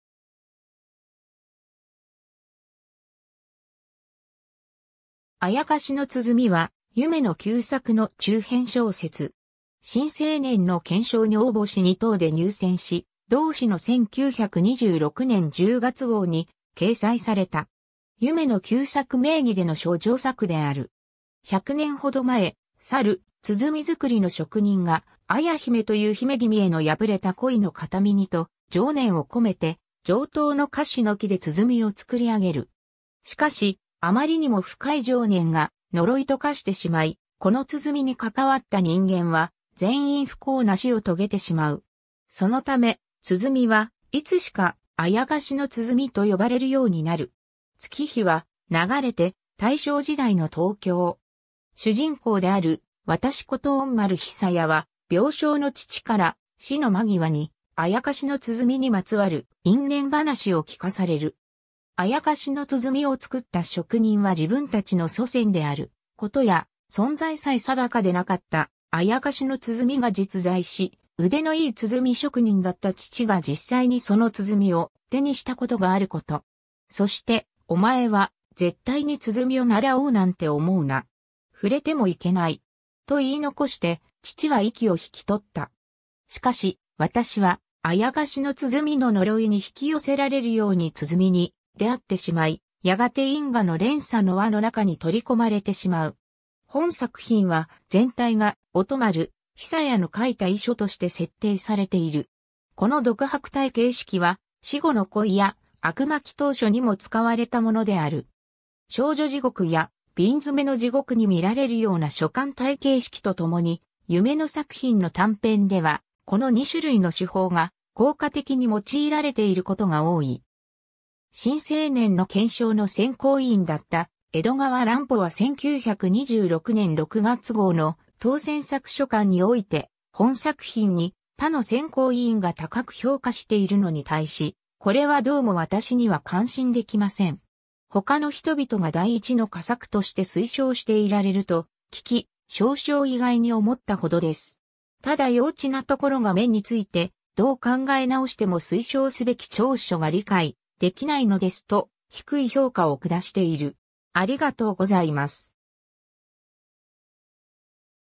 あやかしの鼓
新青年_(日本)
『あやかしの鼓』（あやかしのつづみ）は、夢野久作の中編小説。
『新青年』の懸賞に応募し二等で入選し、同誌の1926年（大正15年）10月号に掲載された。
夢野久作名義での処女作である。
 100年ほど前、さる鼓作りの職人が、綾姫という姫君への破れた恋の形見にと、情念を込めて上等の樫の木で鼓を作りあげる。
しかし、あまりにも深い情念が呪いと化してしまい、この鼓にかかわった人間は全員不幸な死を遂げてしまう。
そのため、鼓はいつしか「あやかしの鼓」と呼ばれるようになる。
 月日は流れて大正時代の東京。
主人公である「私」こと音丸久弥は、病床の父から死の間際に、「あやかしの鼓」にまつわる因縁話を聞かされる。
「あやかしの鼓」を作った職人は自分たちの祖先であることや、存在さえ定かでなかった「あやかしの鼓」が実在し、腕のいい鼓職人だった父が実際にその鼓を手にしたことがあること。
そして「お前は絶対に鼓を習おうなんて思うな。触れてもいけない。」と言い残して父は息を引き取った。
しかし、私は「あやかしの鼓」の呪いに引き寄せられるように鼓に出会ってしまい、やがて因果の連鎖の輪の中に取り込まれてしまう。
 本作品は、全体が音丸久弥の書いた遺書として設定されている。
 この独白体形式は、『死後の恋』や『悪魔祈祷書』にも使われたものである。
『少女地獄』や『瓶詰の地獄』に見られるような書簡体形式とともに、夢野作品の短篇ではこの2種類の手法が効果的に用いられていることが多い。
 『新青年』の懸賞の選考委員だった江戸川乱歩は1926年（大正15年）6月号の「当選作所感」において、本作品に他の選考委員が高く評価しているのに対し、「これはどうも私には感心出来ません。他の人々が第一の佳作として推奨していられると聞き、少々意外に思った程です。」「ただ幼稚な所が目について、どう考えなおしても推奨すべき長所が理解出来ないのです」と、低い評価を下している。